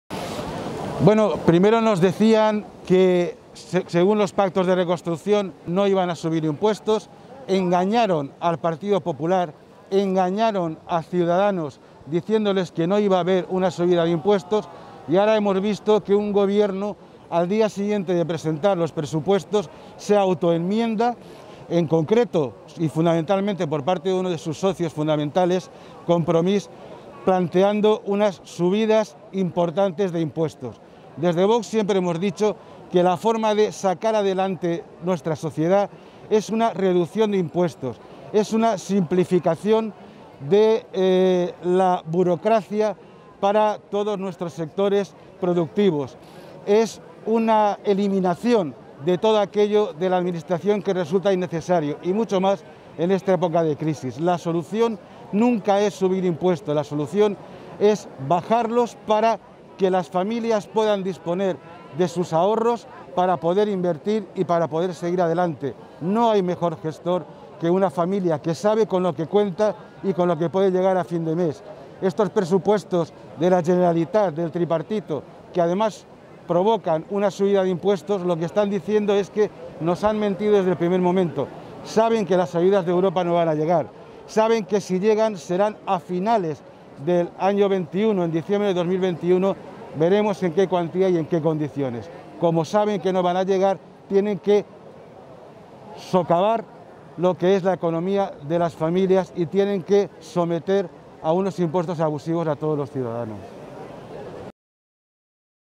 Llanos (VOX) destapa la mentira de los presupuestos del Botánico
💰 José María Llanos destapa el engaño a los valencianos en los presupuestos del Botánico:

"Según los pactos de reconstrucción, el Botánico dijo que no iba a subir impuestos. Pues bien, han presentado una reforma tributaria que plantea una subida importante de impuestos".